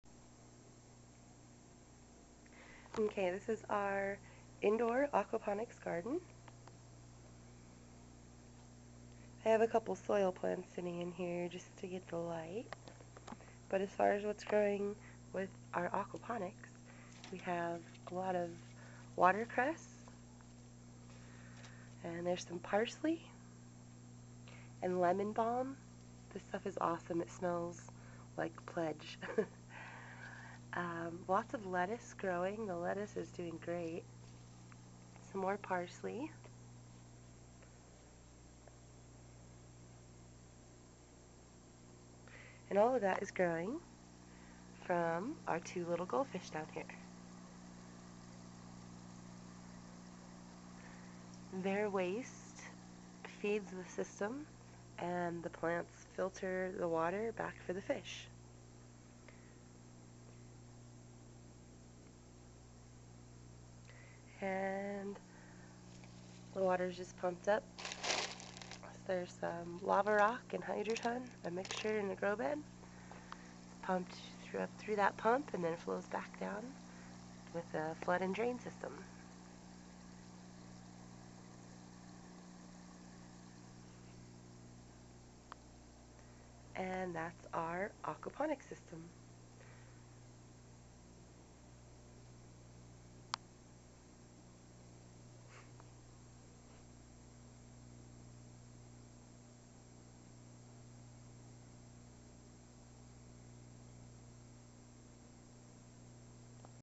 Indoor Aquaponics Garden - Free Food!
This indoor aquaponics garden is growing watercress, lettuce, parsley and lemon balm all with the waste of just TWO gold fish!  The plants in the hydroponic grow bed, which is filled with hydroton and lava rock, filter the water before it returns to the fish in the reservoir.